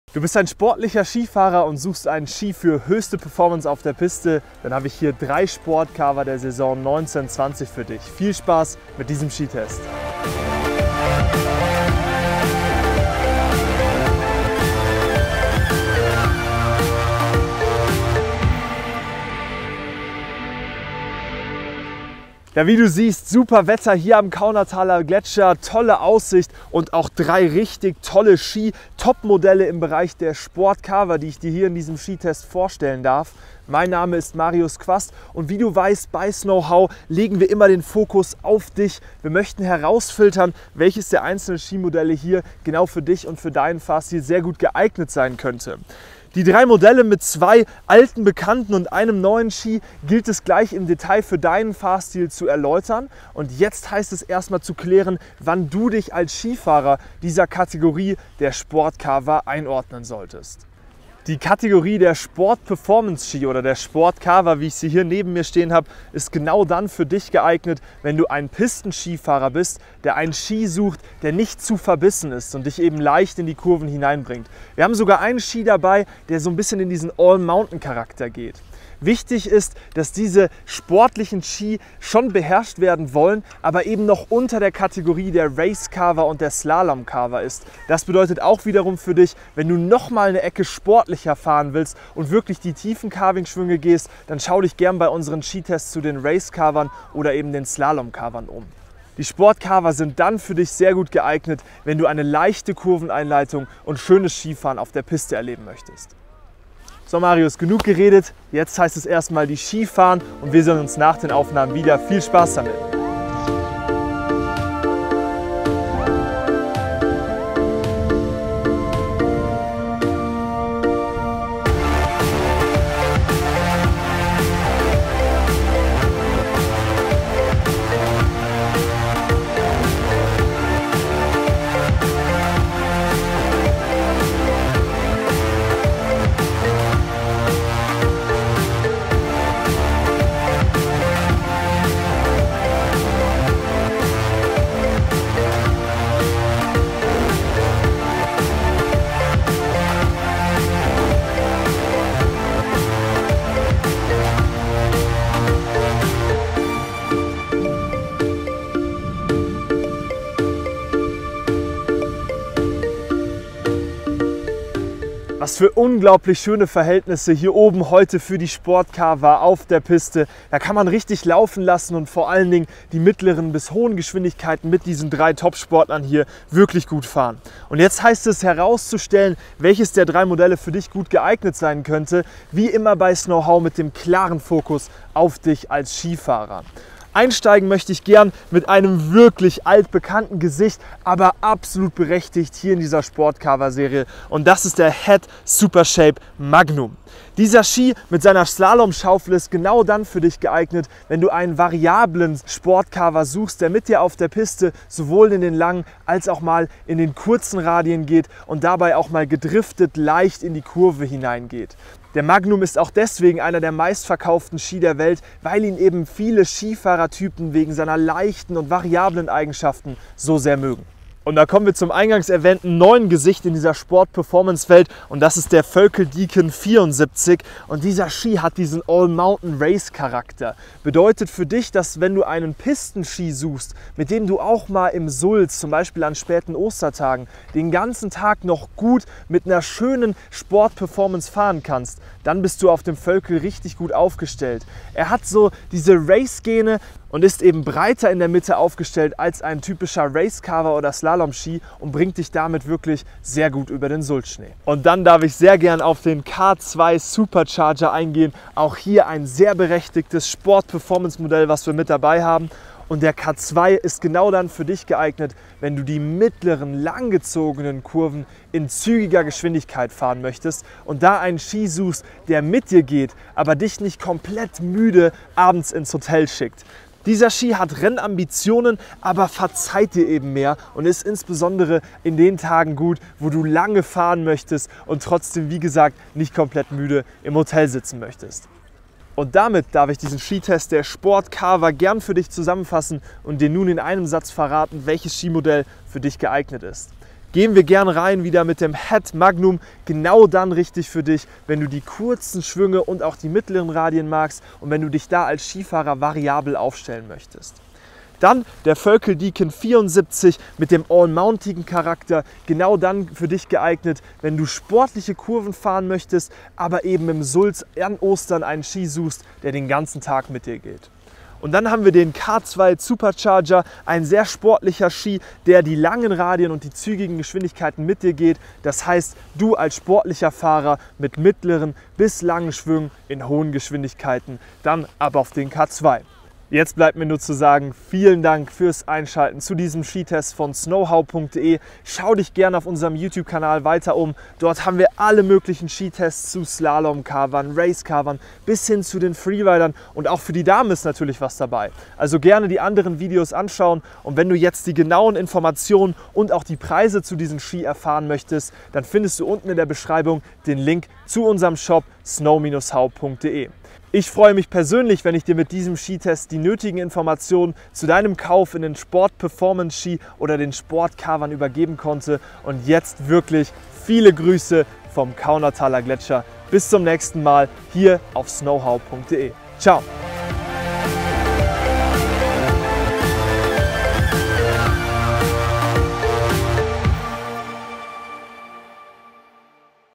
SKITEST: SportCarver | HEAD Supershape i.Magnum - K2 Super Charger - VÖLKL Deacon 74
Mit diesen SportCarver Skimodellen bist du in der Skisaison 2019/2020 sehr gut ausgestattet! Im Skitest verraten wir dir, welcher Ski genau der richtige für dich sein könnte.

► Hast du Anregungen? Schreib’ gern einen Kommentar! 📫

Die Sportcarver, Sport Performance Ski oder auch Crossover Ski eignen sich insbesondere gut für dich, wenn du einen Ski für verschiedene Zwecke besitzen möchtest. So vereinen die Modelle Eigenschaften von Slalomcarver, Racecarvern oder auch Allmountainski miteinander. Das bedeutet für dich, dass eine Variation am Berg sehr gut möglich ist. Ebenso sind die Sportcarver nicht so verbissen wie die rennorientierten Ski, was eine einfachere Schwungeinleitung und komfortablere Tage am Berg bedeutet.

Der HEAD Supershape i.Magnum ist eines der meistverkauften Skimodelle der Welt und das nicht ohne Grund: Der Ski ist variabel aufgestellt, was dir die Kurveneinleitung bei kurzen und leichten Schwüngen sehr vereinfacht. Mit dem Völkl Deacon 74 kannst du auch bei schwererem Schnee sportlich unterwegs sein: Seine Mittelbreite erlaubt auch tolle Carvingschwünge an späteren Ostertagen. Der K2 Super Charger ist ein Sportcarver, der es dir erlaubt, sportliche Kurven in hohem Tempo mit viel Spaß auf der Piste zu fahren, ohne dabei sehr viel Kraft aufzuwenden.

Welchen dieser drei SportCarver würdest du dir kaufen? Wir freuen uns, dir mit diesem Test bei deiner Kaufentscheidung zu helfen.

Hast du schon folgende Skitests gesehen?

So kannst du uns erreichen:

► Vor Ort:
SPORTSWORLD Lingen
Am Darmer Bahndamm 1
D-49808 Lingen/Ems